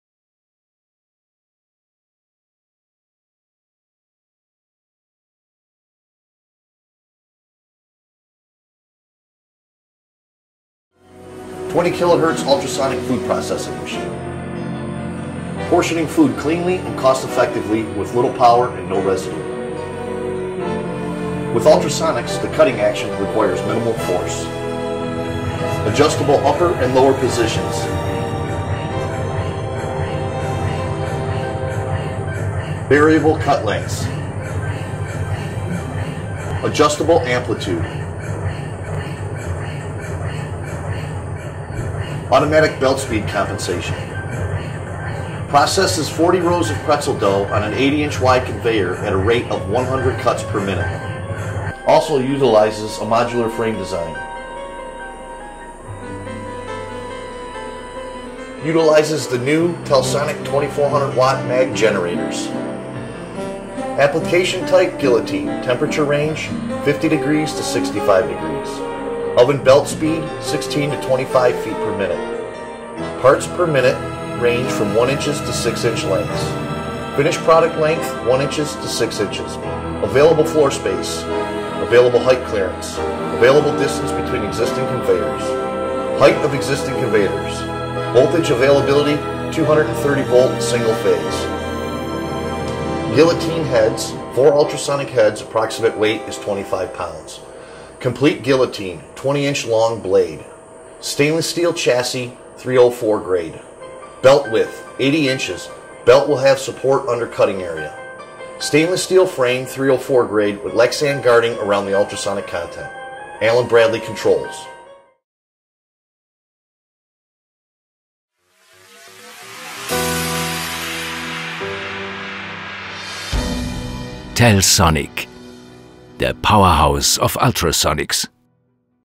Módulo de corte ultrasónico para alimentos
Corte de alimentos, corte en porciones de productos acabados: una cuchilla vibratoria por ultrasonidos trocea el producto con el mínimo esfuerzo, sin dejar residuos y de forma rápida y precisa.
El ultrasonido se aplica desde hace años para cortar productos acabados. En productos de panadería, barritas energéticas, queso, pizza, etc., el sonotrodo vibratorio (frío) reduce la resistencia durante el proceso de corte y se desprende automáticamente de los restos del producto.
Resultado: superficies de corte lisas y reproducibles sin deformación ni daño térmico en los productos.